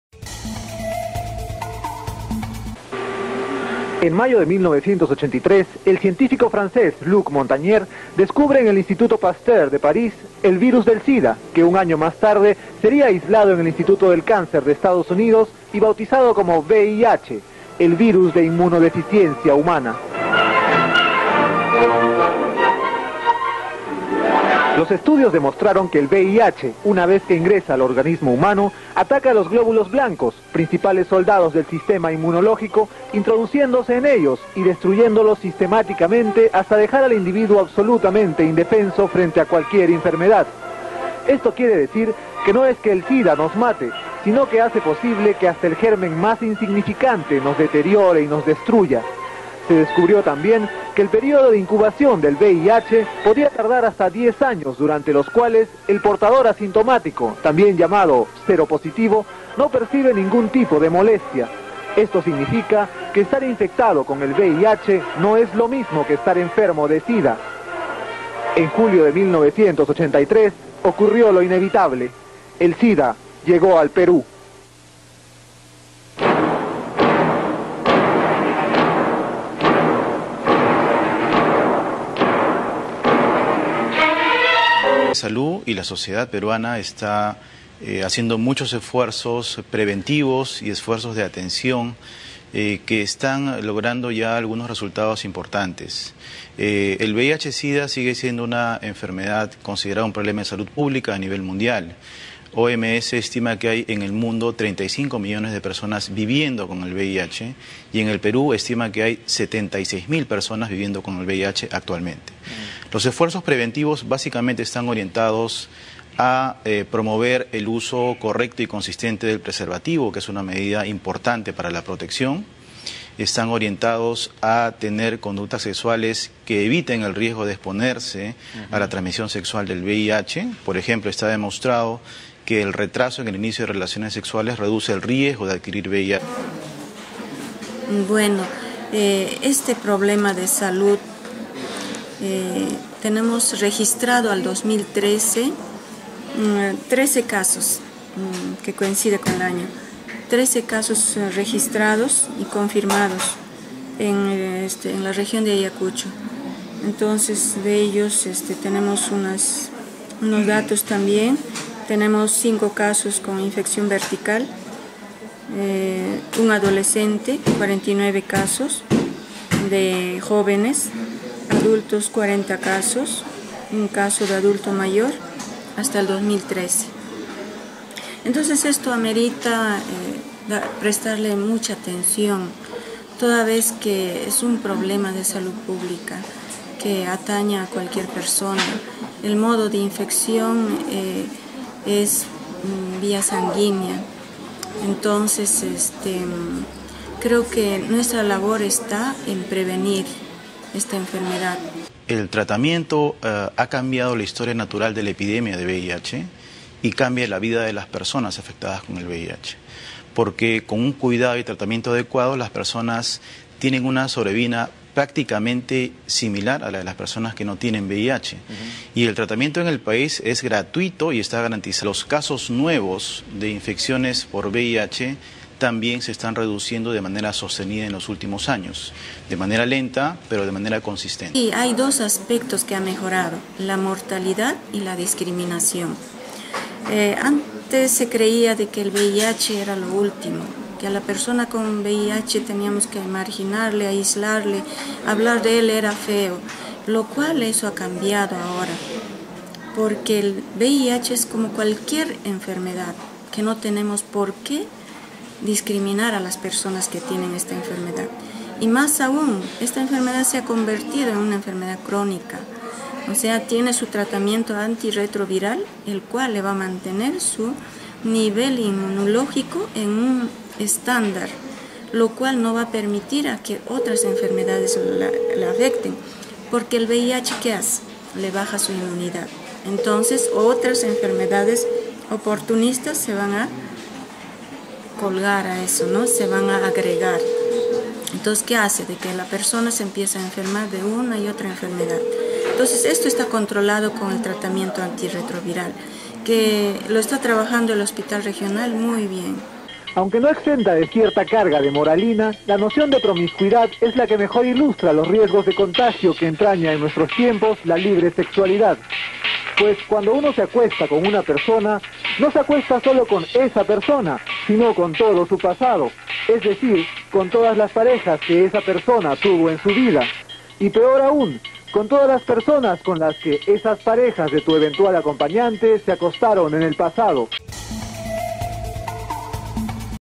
VIH SIDA EN AYACUCHO
El VIH y el SIDA sigue siendo uno de los grandes males que amenaza particularmente a la juventud. ¿Cómo ha venido evolucionando esta enfermedad en el País y en la región Ayacucho?. El reportaje con el apoyo de otras producciones trata de abordar el tema con el propósito de hacer mayor conciencia sobre esta enfermedad.